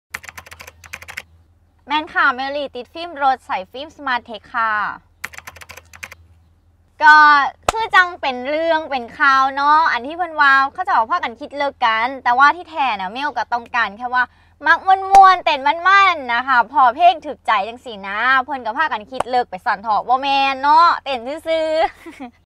ฟังจากปากเมลล์ลี่พริตตี้บ้านนา
เน็ตไอดอลคนดัง เมลล์ลี่พริตตี้บ้านนา ใช้ฟิล์มสมาร์ทเทค ดำนอก สว่างใน จนคนเข้าใจผิด

สามารถหาซื้อฟิล์มกรองแสงสมาร์ทเทค,ซันการ์ด และติดตั้งได้ที่ร้านประดับยนต์ชั้นนำทั่วประเทศ สอบถามเพิ่มเติมได้ที่ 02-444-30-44
Line : @smarttecfilm